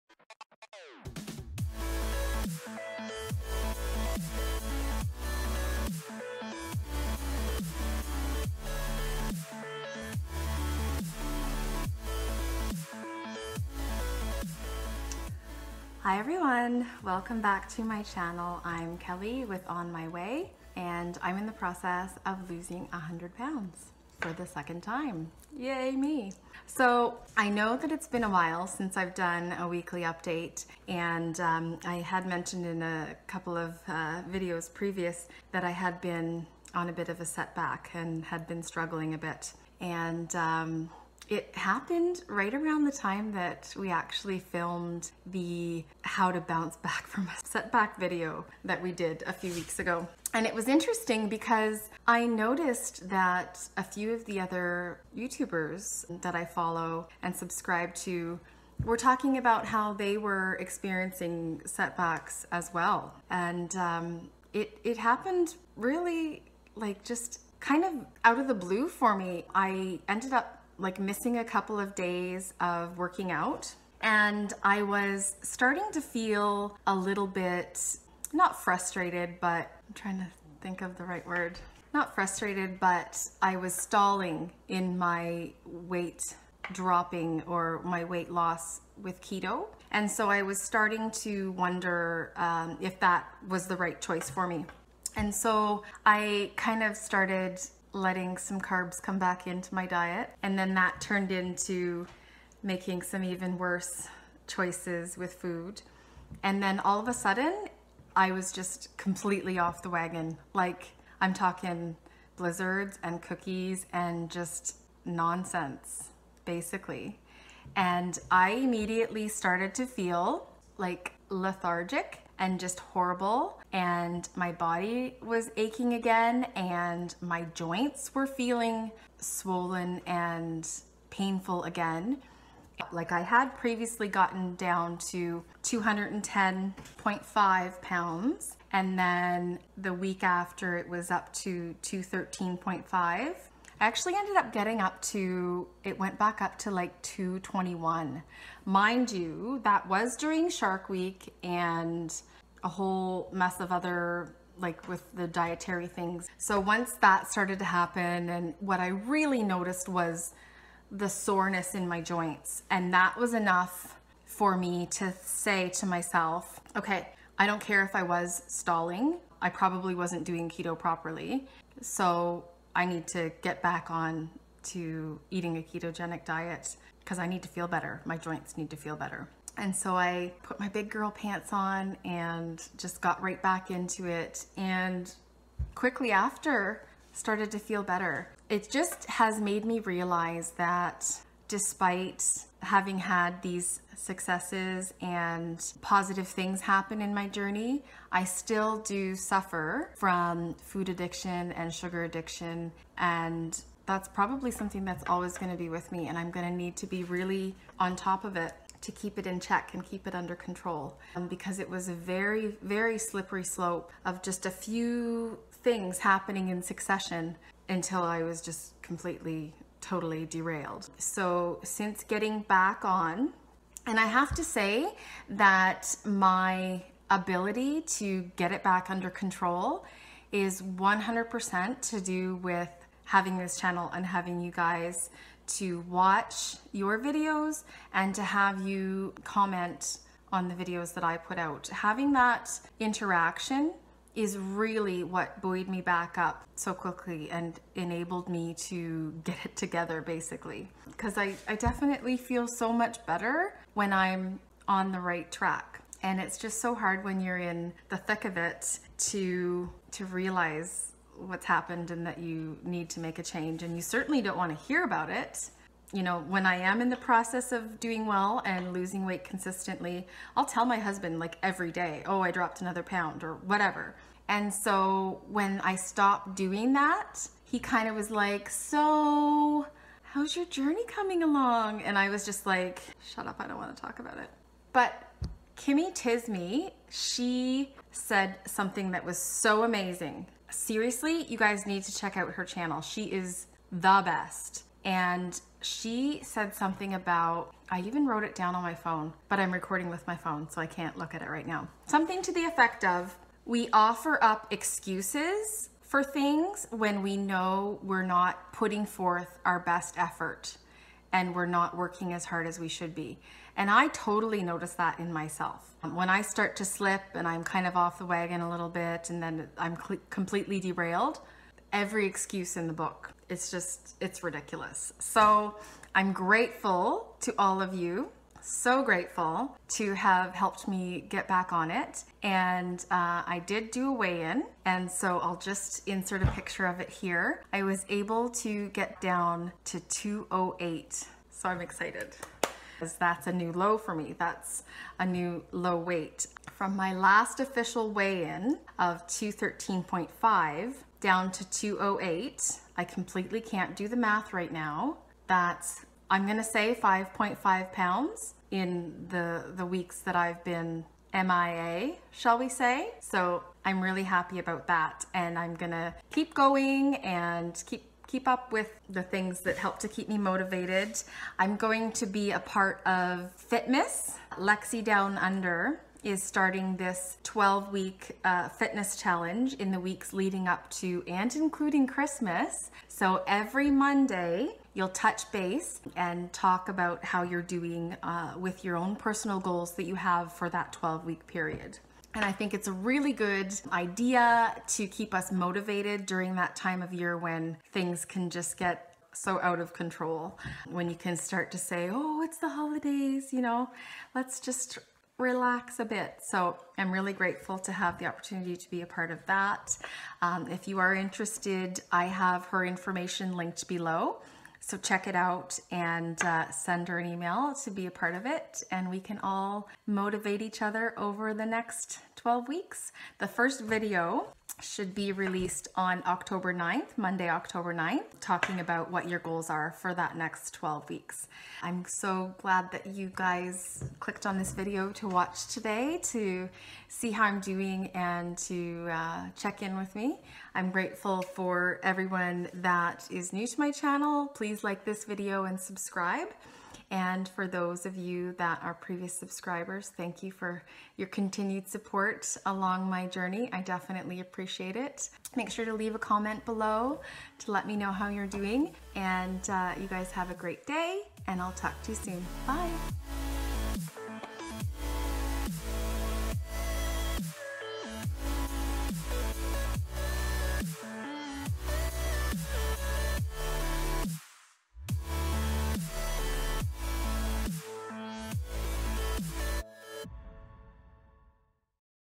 Weighing In After a Setback || Weekly Update - Oct 4, 2017 (My 100lb Weight Loss Journey)
I've been MIA - I talk about why - and the after-effects of being off the wagon for a while.

I'm Kellie - and I'm over half way through losing 100lbs - again! This time, I'm doing Intermittent Fasting & Keto. So follow along & watch me as I go! Your support on my journey is very much appreciated! ***